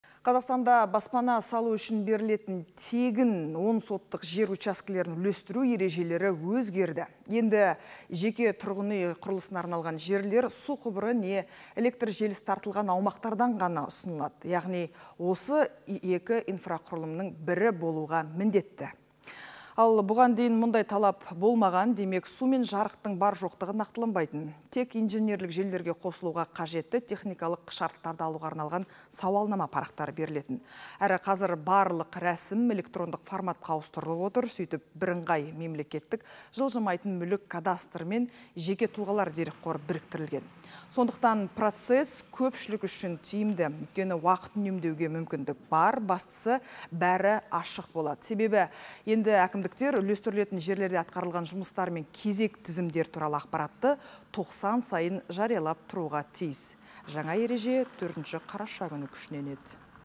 Мемлекеттен тегін берілетін 10 сотық жерді үлестіру ережелері өзгерді
Қазақстанда баспана салу үшін берілетін тегін 10 сотық жер учаскелерін үлестіру ережелері өзгерді. Енді жеке тұрғын үй құрылысына арналған жерлер су құбыры не электр желісі тартылған аумақтардан ғана ұсынылады. Яғни, осы екі инфрақұрылымның бірі болуға міндетті.  Ал бұған дейін мұндай талап болмаған. Демек, су мен жарықтың бар-жоқтығы нақтыланбайтын. Тек инженерлік желілерге қосылуға қажетті техникалық шарттарды алуға арналған сауалнама парақтары берілетін. Әрі қазір барлық рәсім электрондық форматқа ауыстырылып отыр. Сөйтіп, бірыңғай мемлекеттік жылжымайтын мүлік кадастры мен «Жеке тұлғалар» дерекқоры біріктірілген. Сондықтан, процесс көпшілік үшін тиімді. Өйткені, уақыт үнемдеуге мүмкіндік бар. Бастысы, бәрі ашық болады. Себебі, енді әкімдіктер үлестірілетін жерлерде атқарылған жұмыстар мен кезек тізімдері туралы ақпаратты тоқсан сайын жариялап тұруға тиіс. Жаңа ереже 4 қараша күні күшіне енеді.